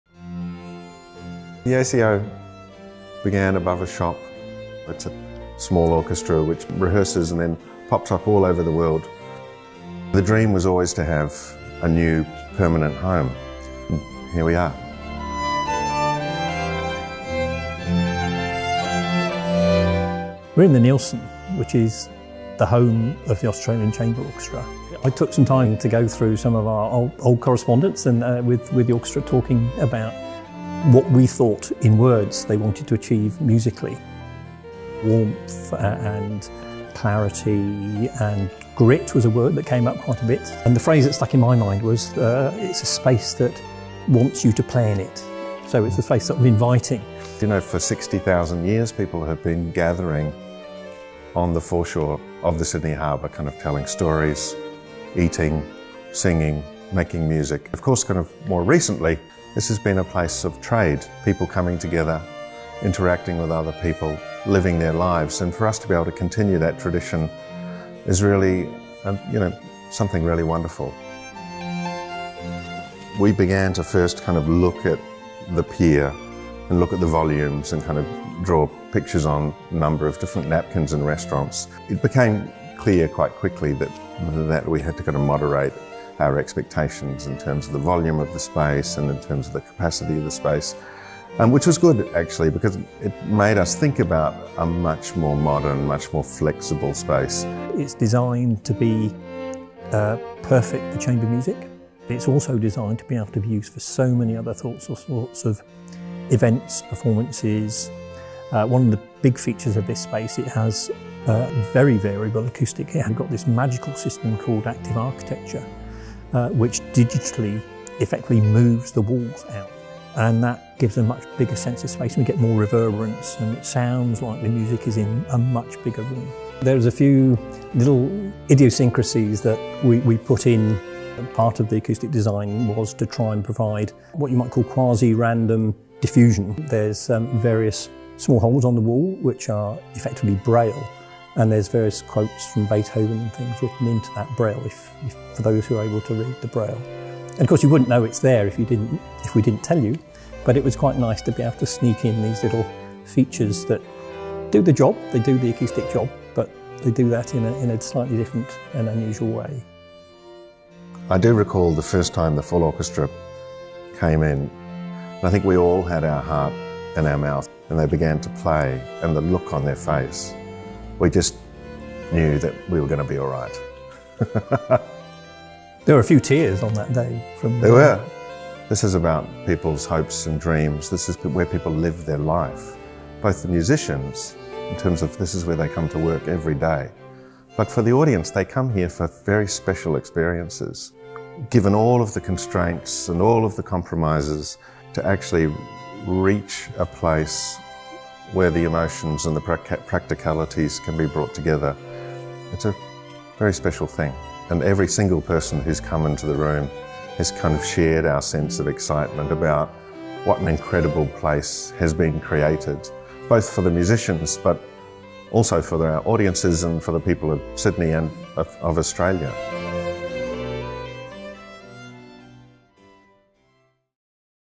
The Story of ACO On The Pier | ARUP Designers | Australian Chamber Orchestra
The making of ACO Pier 2/3, our new home from where we create, broadcast and perform our music for you across Australia and the world. ✨